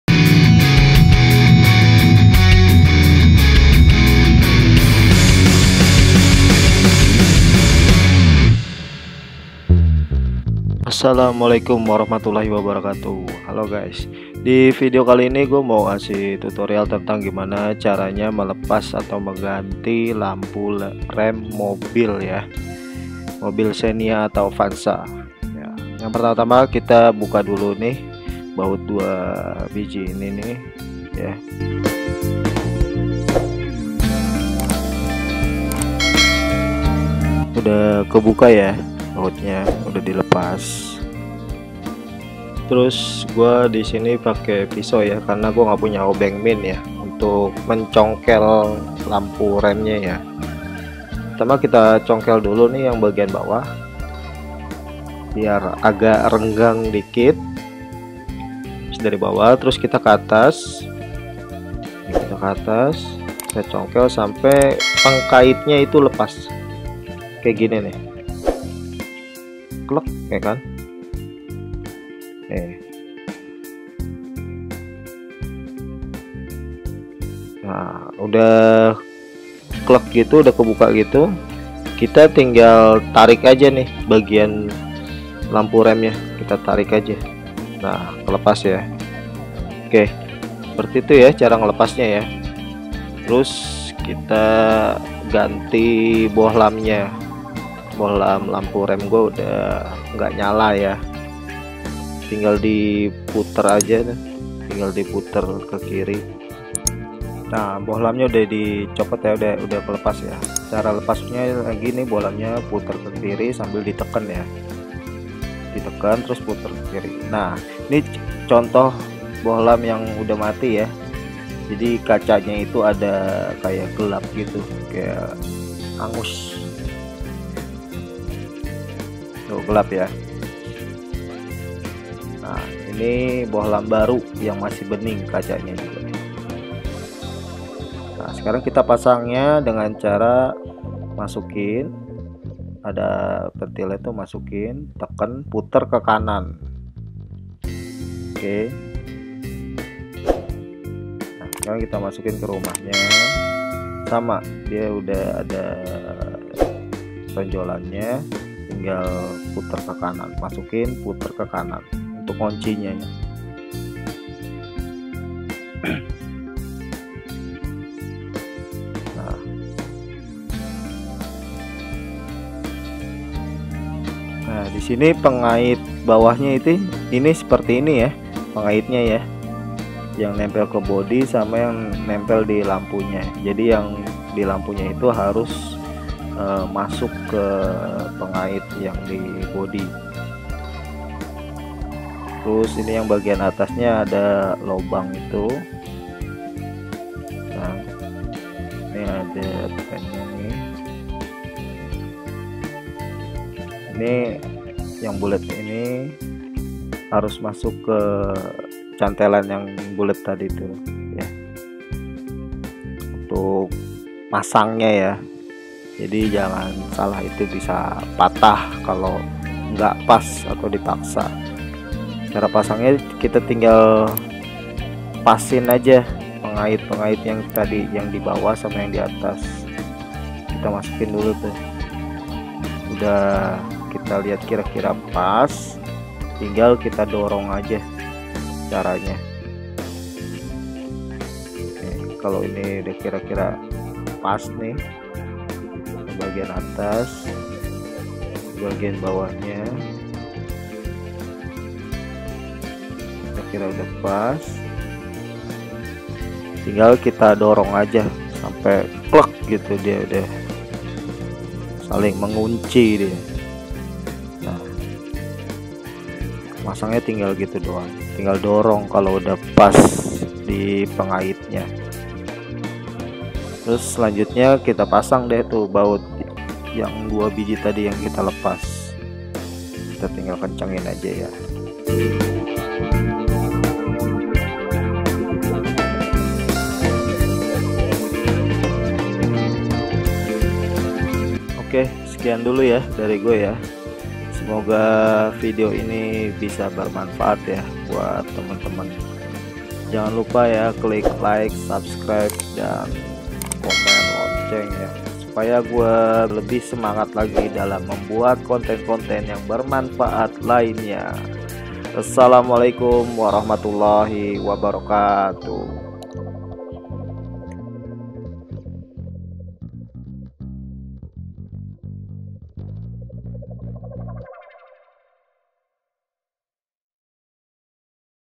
Cara Melepas Dan Memasang Lampu Rem Mobil XENIA - AVANZA
Cara Melepas Mengganti Memasang Lampu Rem Mobil XENIA atau AVANZA yang mati,
biasanya lampu rem itu mati dikarenakan faktor usia lampu, dan berapa seringnya mobil itu dipakai,
sok monggo disimak kalo mau tahu cara mengganti lampu rem mobil avanza atau xenia

Semoga bermanfaat,

mobil xenia
mobil avanza
daihatsu
toyota
nissan
suzuki